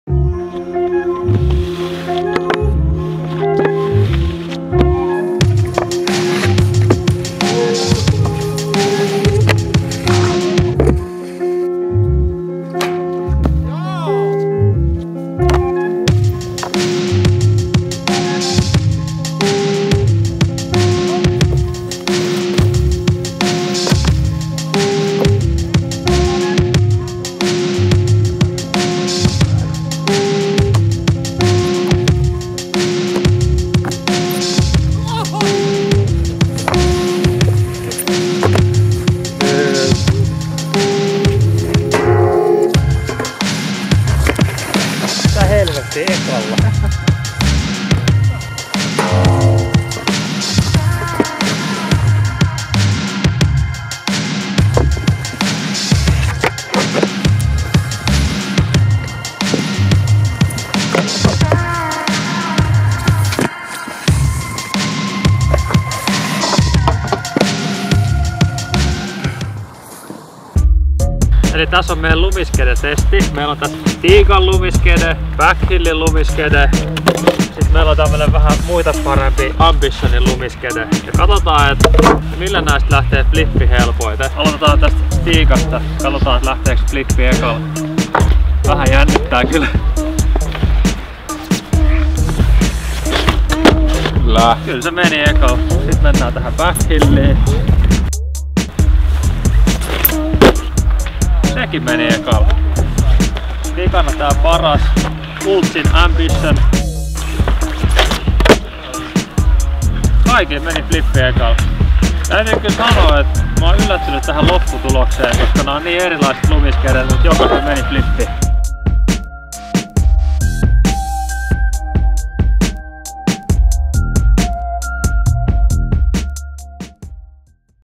Lumiskedespesiaali | Leppävaaran skeittipuisto, Salmisaari ja lumiskedetesti
Lumiskedespesiaalissa käydään Leppävaaran skeittipuistossa, Salmisaaren spotilla ja lopuksi flippitestissa kuuluisa STIGA lumiskede vs. Backhill vs. Ambition! Kiitos vetäjille:
Ultsi, Eetu @eetutielinen, Hemmo @oghuima, Piispa @fakinhipster ja Mevert @mevert
Taustalla soi meidän Hemmon itse tekemät biisit. ks. Spotify (421beats)

Laita kanava ihmeessä tilaukseen niin saat tiedon tulevista videoista :)